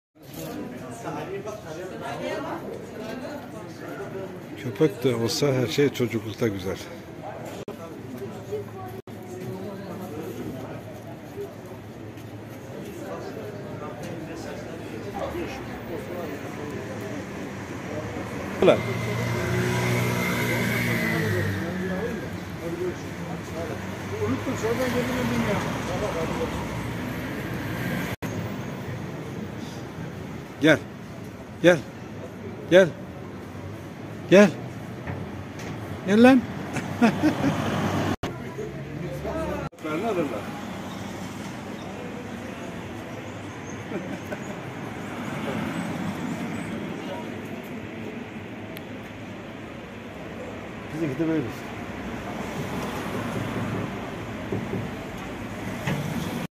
Miniklerin dostluğu
Hayatta herşey çocukluk yıllarında güzel Sokak köpeklerinin şakalaşması ve ödül sunumu